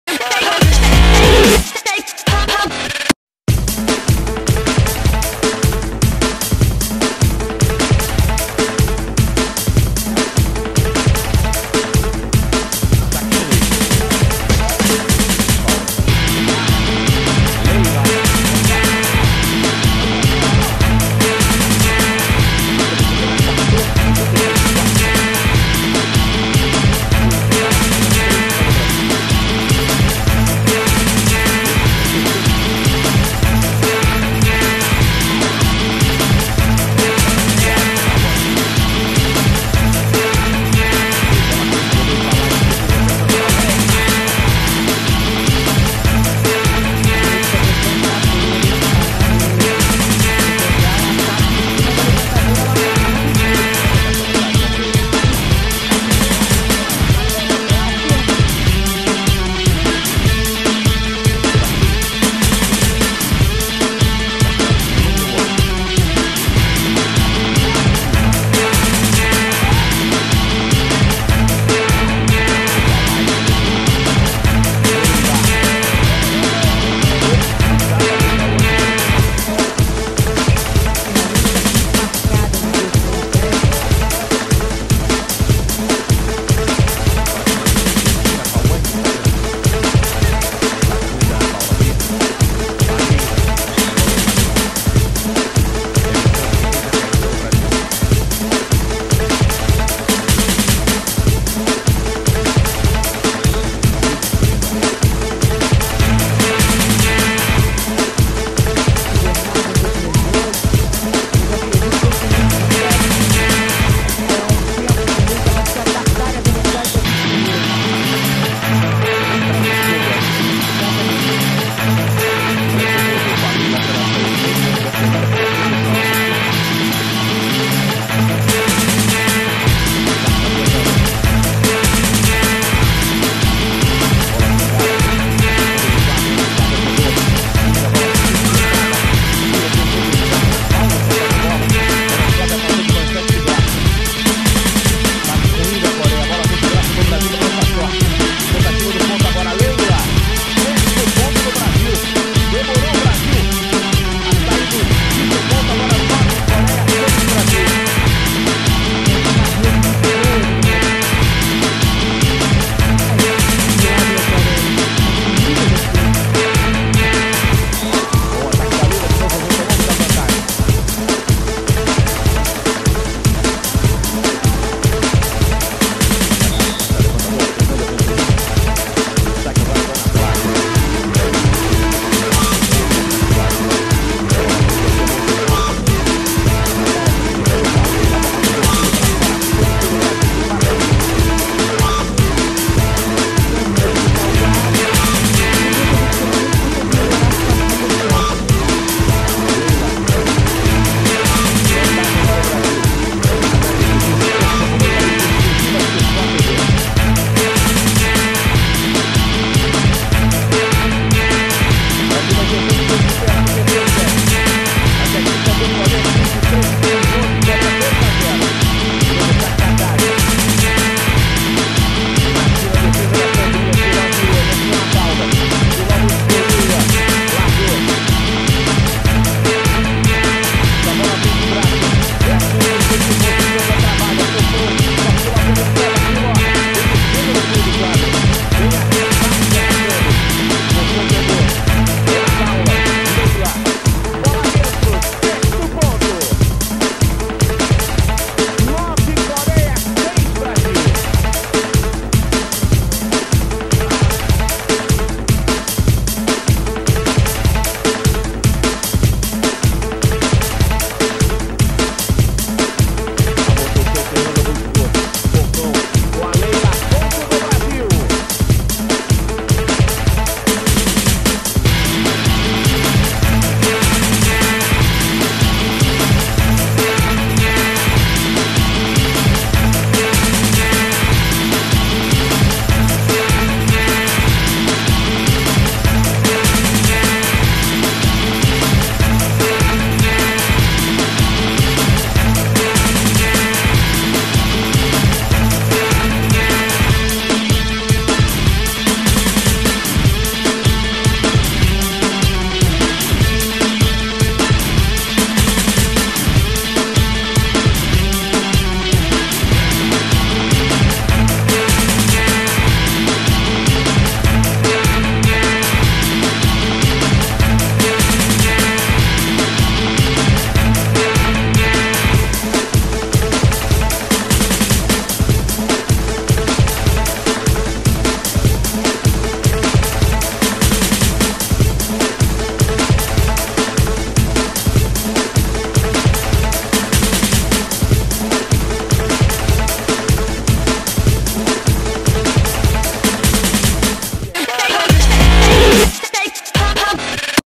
【The pretty spiker from Team Brazil - Leila Barros!✨】強烈な左利きスパイクと眩しい笑顔 - レイラ・バロス ! 🇧🇷
巴西女排嘅甜美殺手！🤩
The pretty spiker from Team Brazil - Leila Barros!✨
強烈な左利きスパイクと眩しい笑顔 - レイラ・バロス ! 🇧🇷